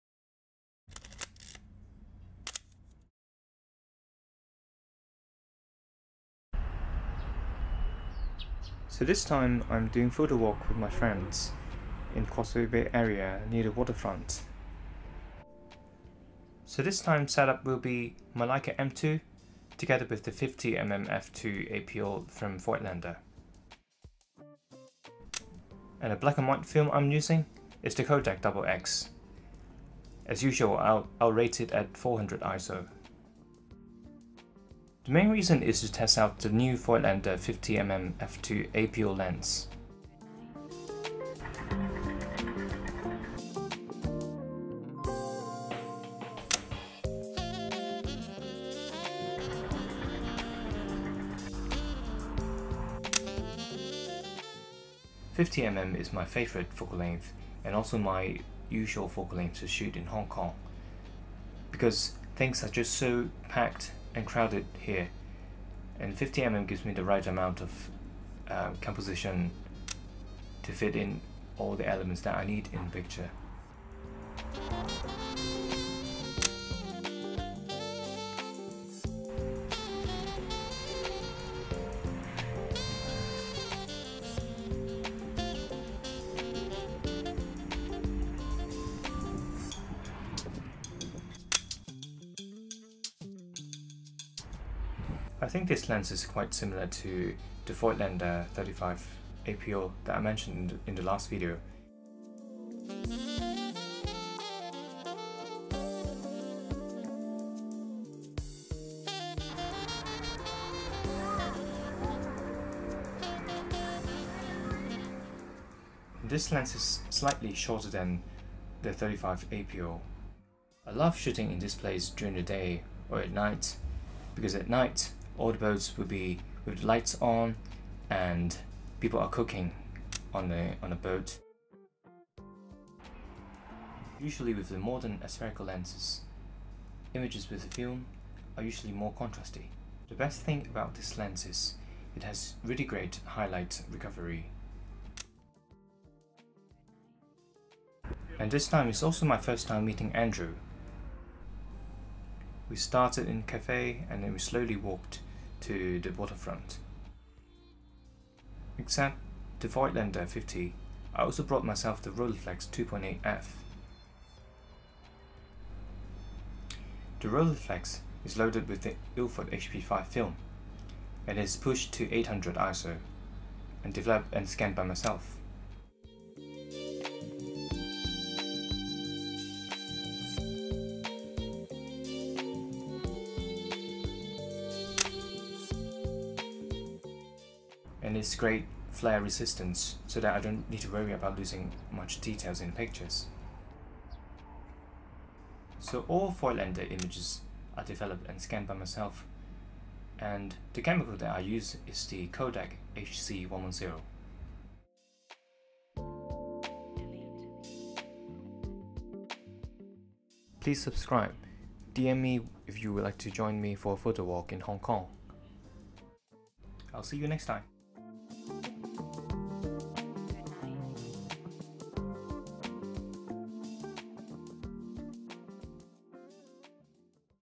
Hong Kong Street Photography - Voigtlander 50mm f2 APO Lanthar and Kodak Double-X Film
Trying this Voigtlander 50mm f2 APO Lanthar with Kodak Double-X black and white film rated at ISO 400 to do some street photography in Causeway bay area in Hong Kong.

Camera: Leica M2
Meter: Voigtlander VCII
Film: Kodak Double-X 250 at 400
Scanner: Epson V800

More content below!